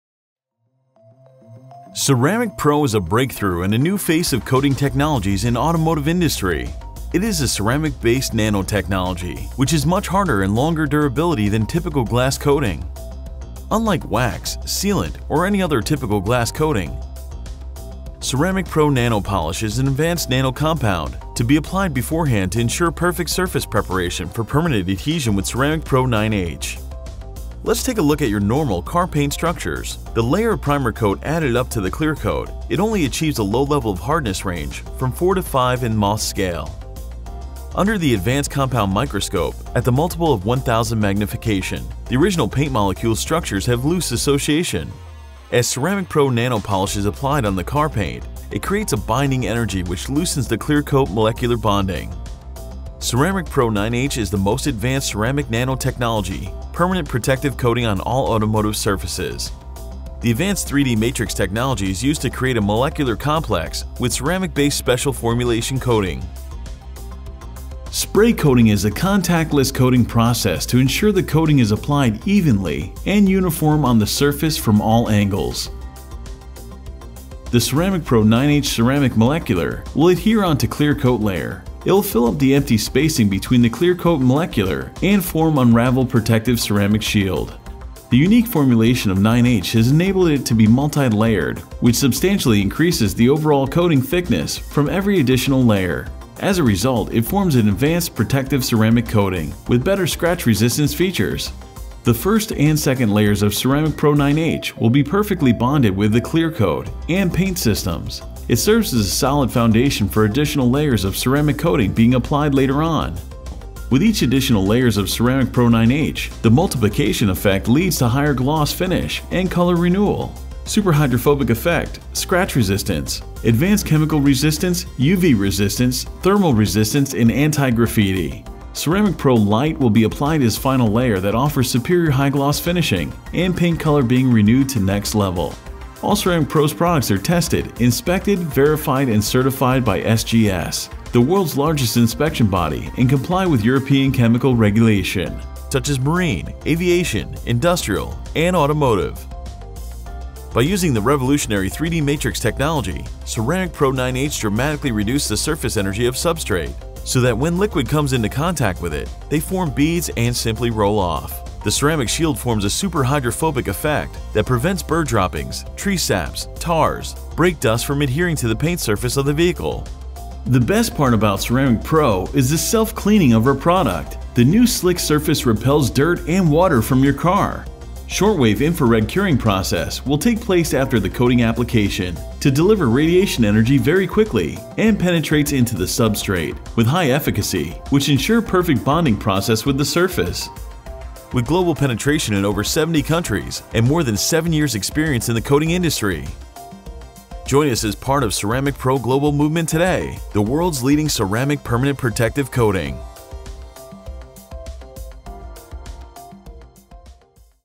Why Ceramic Pro is unique? True nano ceramic coating!
Ceramic Pro 9H is a Permanent Nano-Ceramic Paint Coating that features a High Gloss finish, unmatched Super Hydrophobic Effect, Scratch Resistance, Chemical Resistance, UV Resistance, Thermal Resistance and Anti-Grafitti.

Both the Super Hydrophobic and Anti-Grafiti effect combined mean the surface coated with 9H will stay cleaner for longer as dirt and grime will not stick to the surface and the super hydrophobic effect of the coating will cause water to bead up and roll of the surface with any dirt and grime, the hard ceramic film also offers superior protection from damaging contamination and harsh chemicals.

9H forms a permanent bond to the paint work and will not wash away or break down, 9H can only be removed by abrasion making it a highly durable protective coating to protect your paint work for damaging contaminants.

The unique formulation of 9H has enabled it to be multi-layered which means the thickness of the coating can be increased with additional layers allowing a thicker / harder film that will increase its scratch resistance.

    Permanent Protection
    Scratch Resistance (Above 9H)
    Super Hydrophobic Effect
    Weather & UV Resistance
    Thermal Resistance (up to 750°C)
    Anti-Graffiti
    Advanced Chemical Resistance
    Oxidation & Corrosion Resistant
    High Gloss Finish

Ceramic Pro 9H can only be applied by Approved Applicators.

Ceramic Pro has been officially tested by "SGS" SGS is the world's leading inspection,